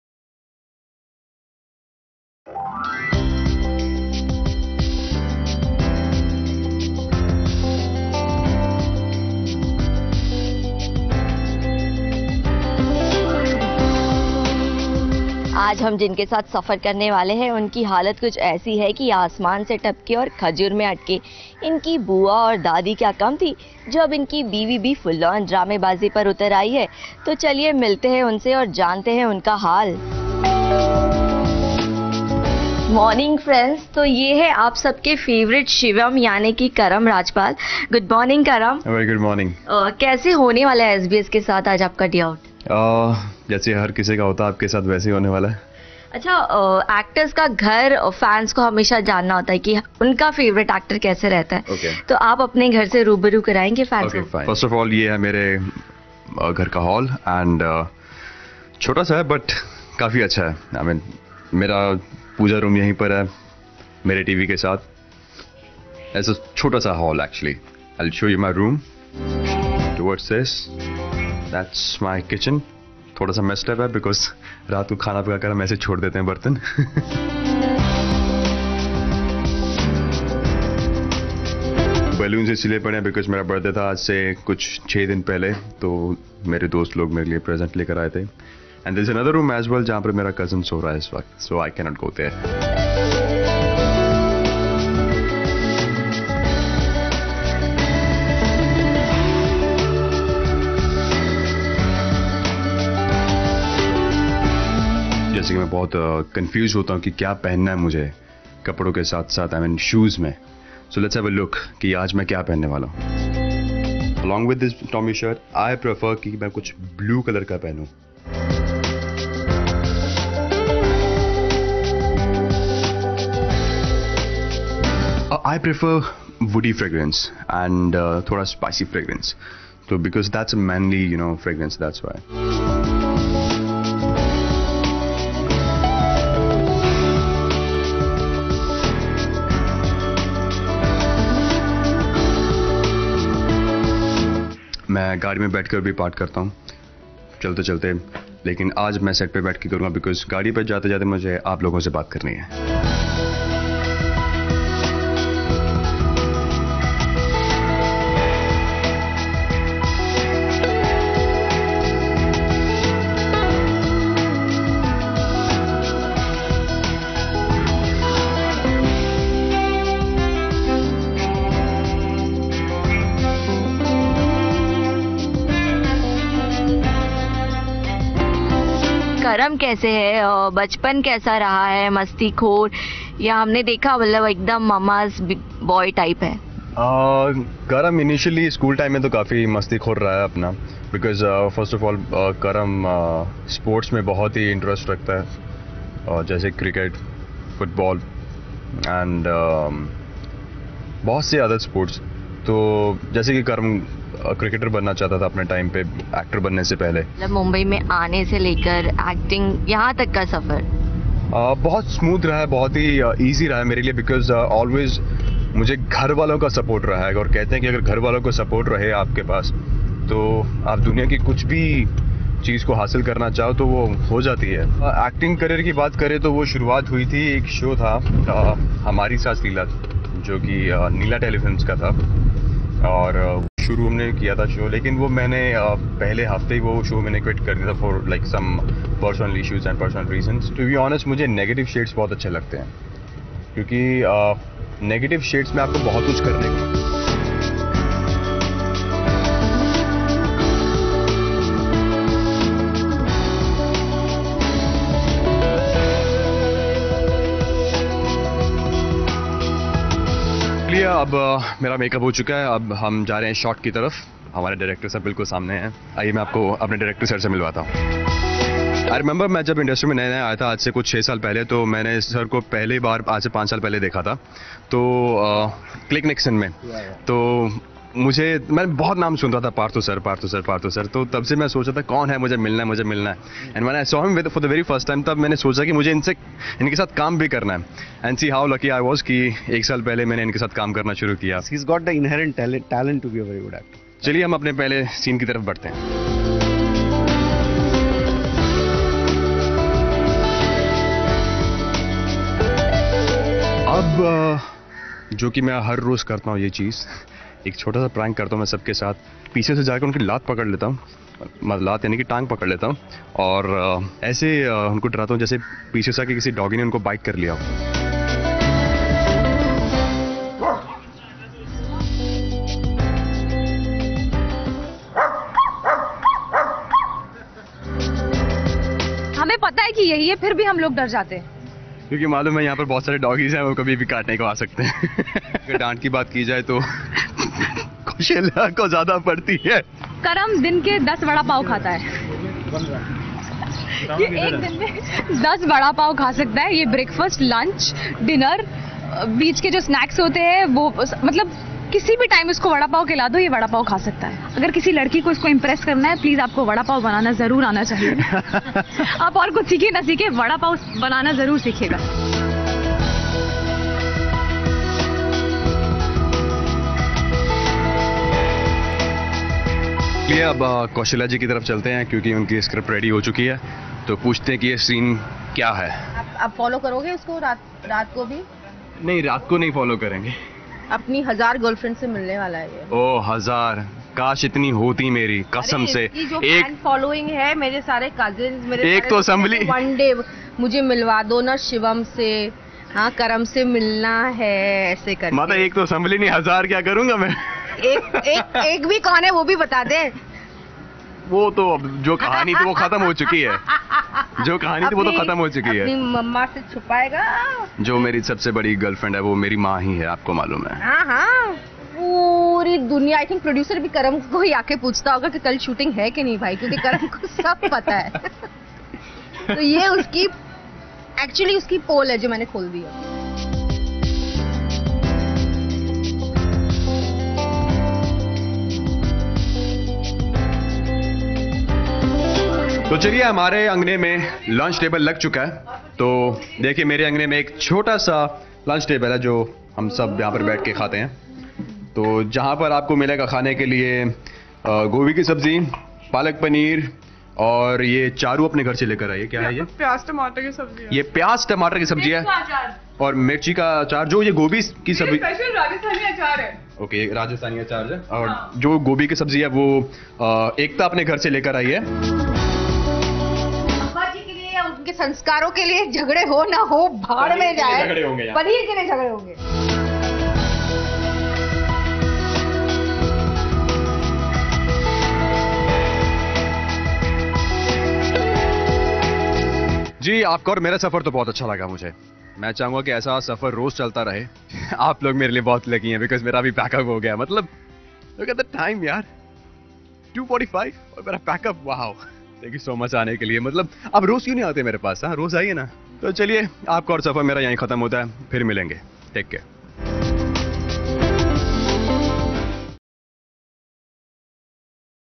A day out with Karam Rajpal aka Shivam of Mere Angne Mein
A day out with the actor Karam Rajpal aka Shiva  of the serial Mere Angne Mein.